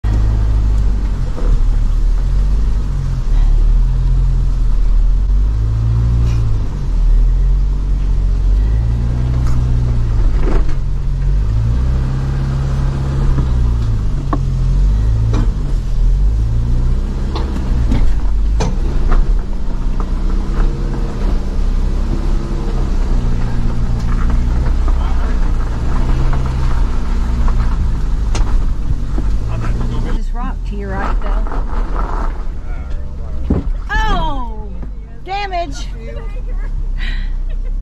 First Ford Bronco 6G to Make it To the Pickett State Park Car Hauler 😎
This is the first ford bronco 6G on record to make it to the Pickett State Park Car Hauler.  If not, show me proof.

Links to Products I Use and Recommend:
Dog Cargo Liner
Car Door Protector
 - Broncbuster Tie Rod BusterBrace Set
 - Broncbuster BroncBushing
 - Broncbuster Rear Shock Skids